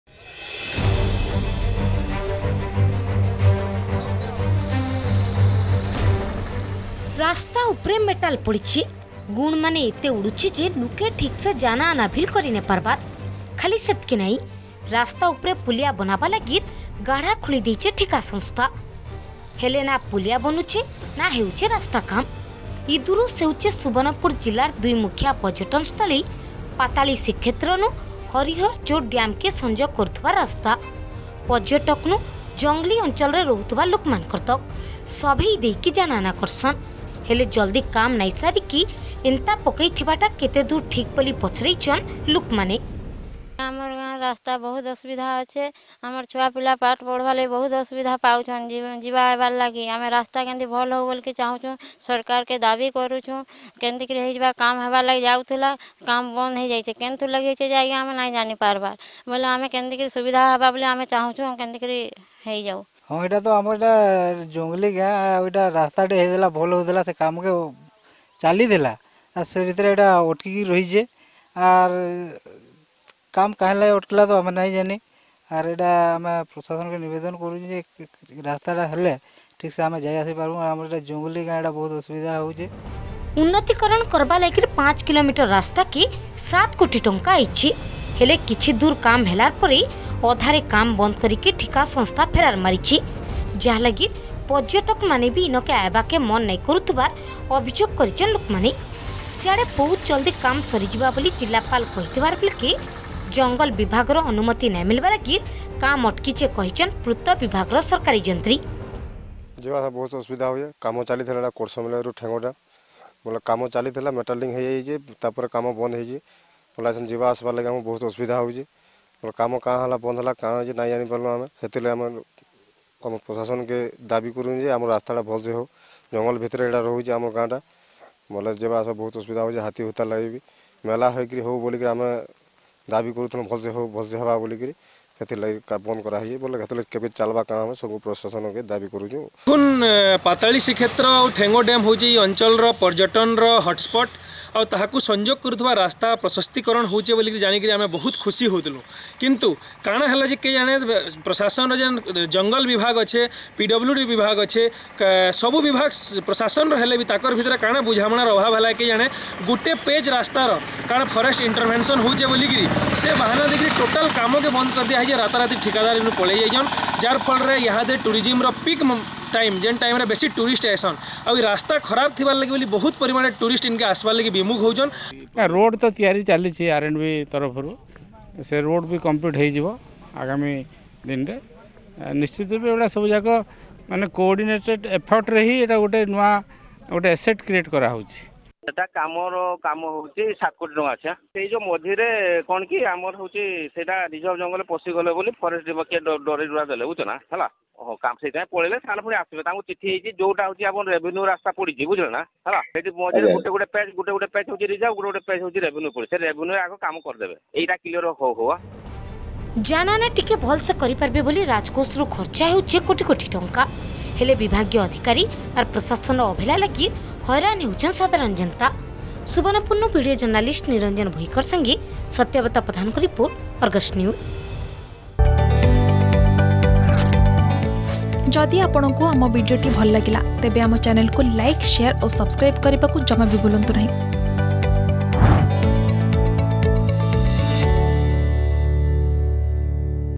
ପାଏନ୍‌ ଥି ଗଲା ୭ କୋଟି ଟଙ୍କାବ || Incomplete Road || Subarnapur
ଯାନା ଆନା ଟିକେ ଭଲସେ କରିପାରବେ ବୋଲି ରାଜକୋଷରୁ ଖର୍ଚ୍ଚା ହେଉଛେ କୋଟି କୋଟି ଟଙ୍କା । ହେଲେ ବିଭାଗୀୟ ଅଧିକାରୀ ଆର୍ ପ୍ରଶାସନର ଅବହେଲା ଲାଗିର୍ ହଏରାନ୍ ସାଧାରନ୍ ଜନତା । ସୋନପୁରେ ୫କିମି ରାସ୍ତା ଲାଗିର୍ ୭ କୋଟି ଟଙ୍କା ଆଏଲା ହେଲେ କିଛି ଦୁର୍ କାମ କରିକି ଠିକା ସଂସ୍ଥା ଫେରାର ମାରିଥିବାର୍ ଅଭିଯୋଗ କରିଛନ୍ ଲୁକେ।